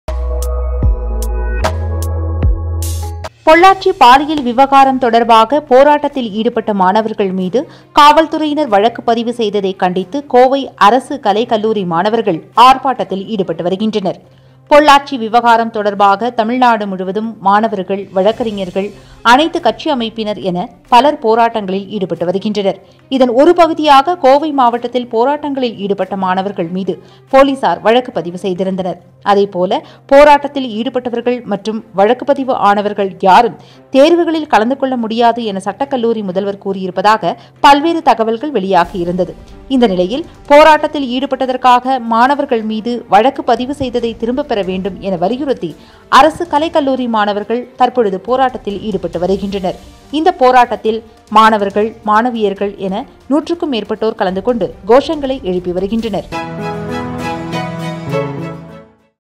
Law college students protest in Coimbatore
Pollachi incident: Law college students in Coimbatore protest against the action taken by police on protesting students.